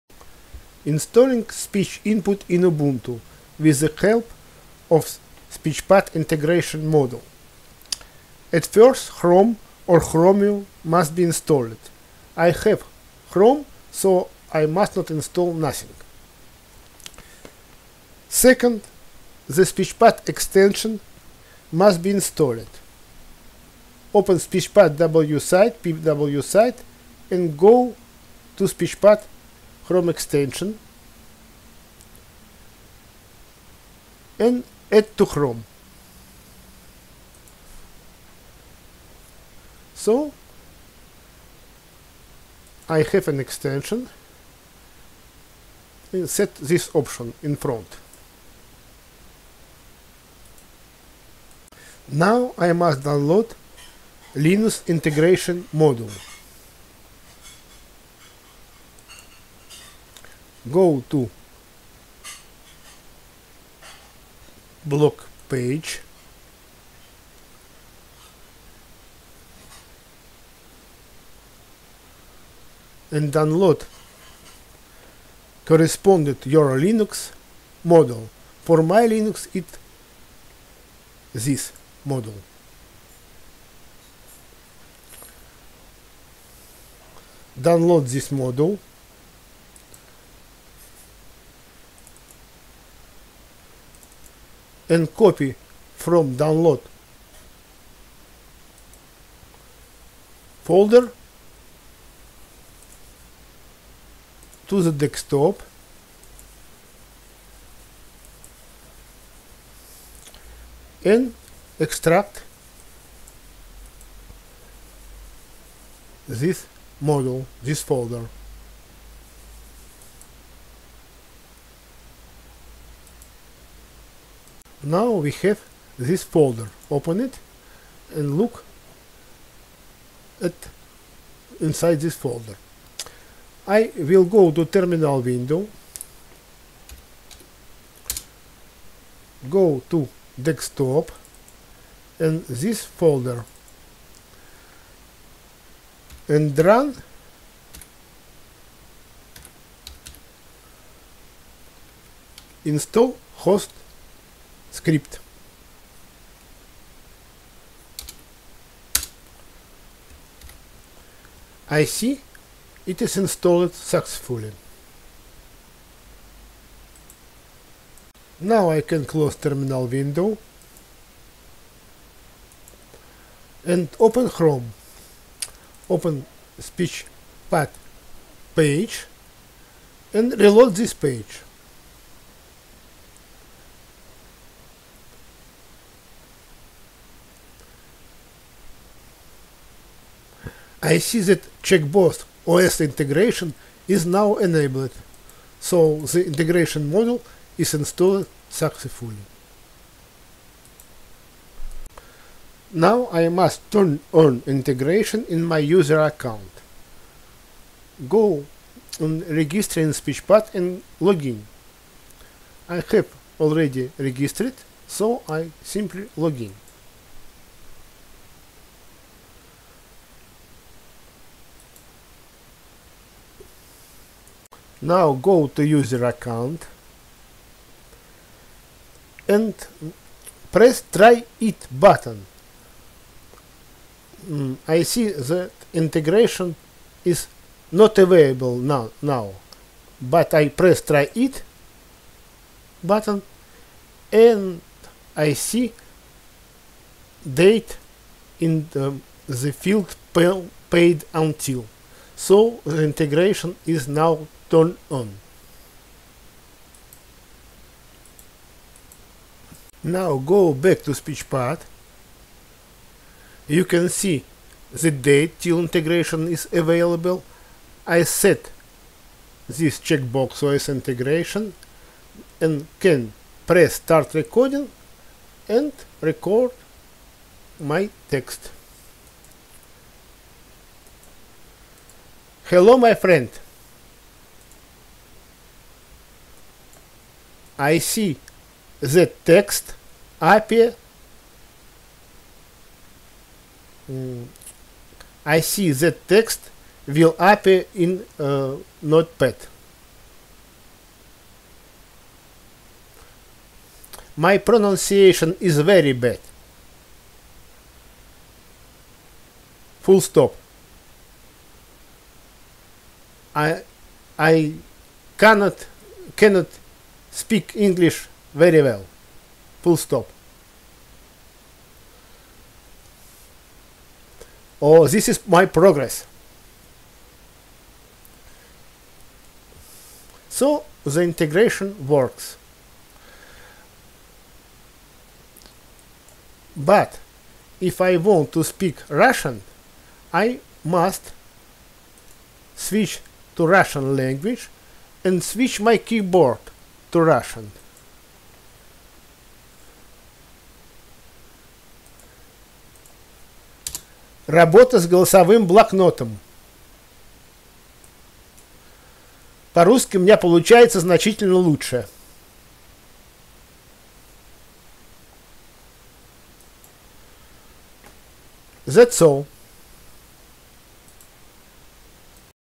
Install speech input in Ubuntu
Install speech input in Ubuntu with the help of Speech Pad Linux integration module
Use Google Speech recognition as the direct voice input in Linux applications.